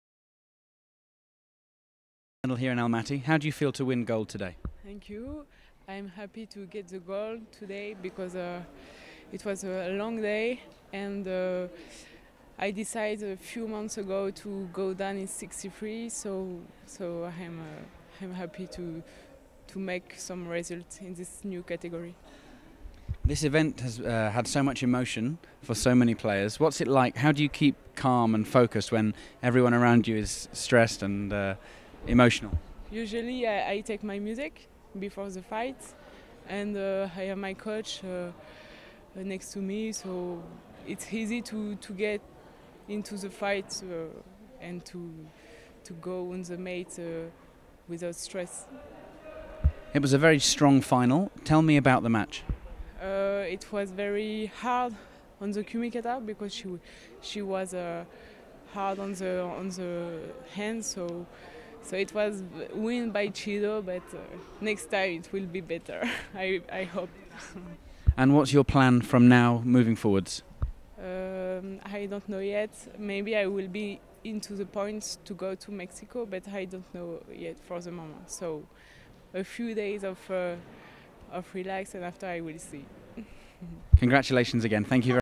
Interview Margaux PINOT (FRA) Winner -63Kg Almaty Judo GP
PINOT beats top seed for French victory
Samsun Grand Prix winner Margaux PINOT (FRA) was on top of a Grand Prix medal podium again as she beat Asian Championships bronze medallist and top seed TSEND-AYUSH Tserennadmid (MGL) in the -63kg final.
PINOT made sure that their head-to-head series would start with an entry in the win column for the Frenchwoman as she prevailed on shido penalties 1:2.